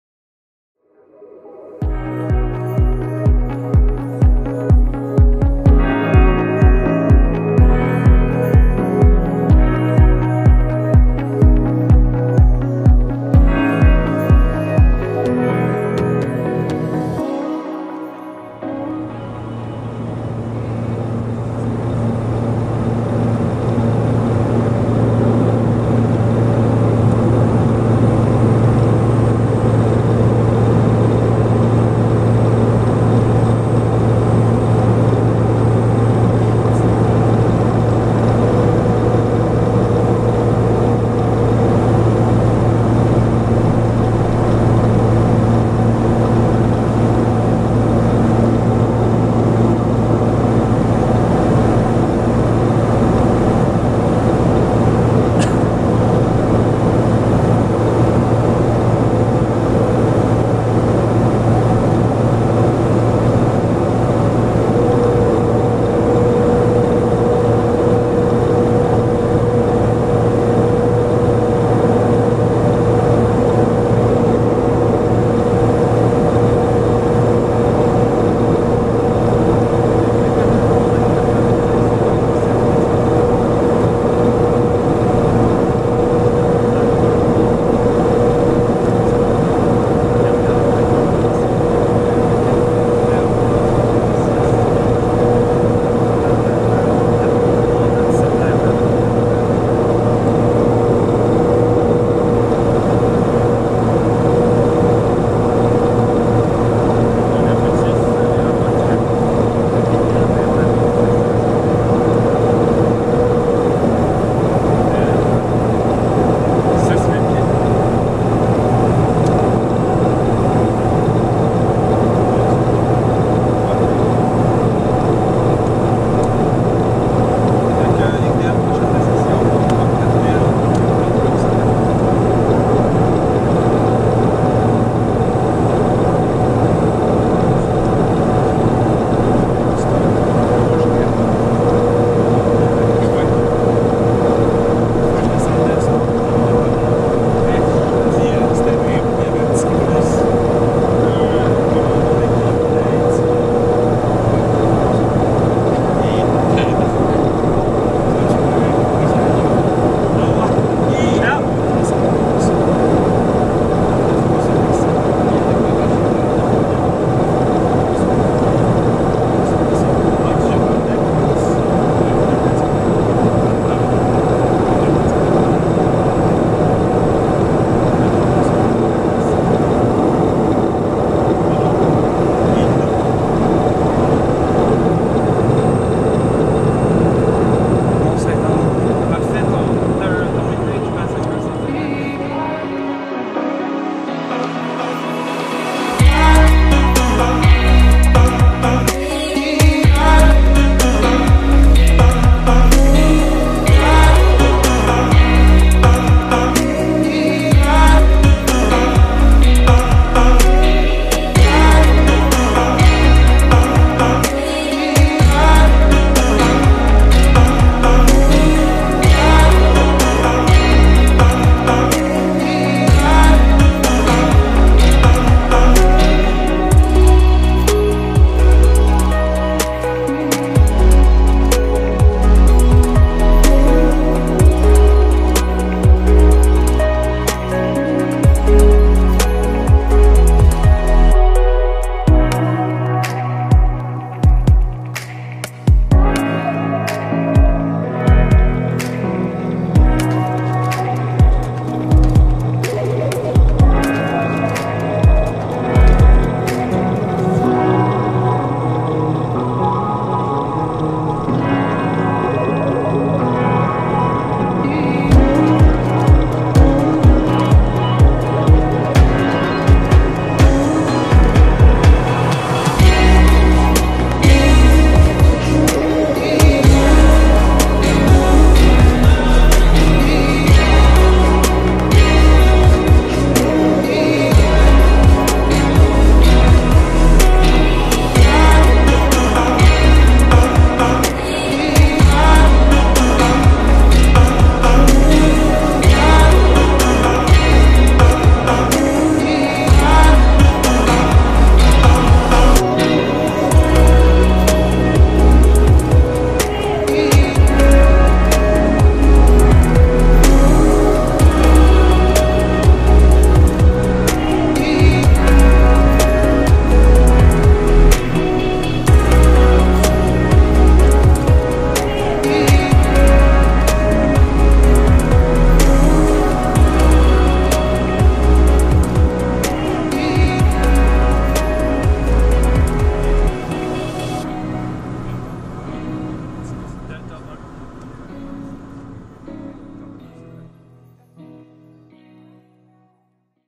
✈️ IFR FLYING | Instrument flying | Bathurst to Quebec city, Canada | YQB | Pilatus PC-12 | 2019
Approach to Quebec city airport

The video was taken from the first officer window of the Pilatus PC-12 whit a GoPro while on an approach to Quebec city airport

Pilatus PC-12/47
F/O : Jimmy Crawford

Pilatus PC-12: The World’s Greatest Single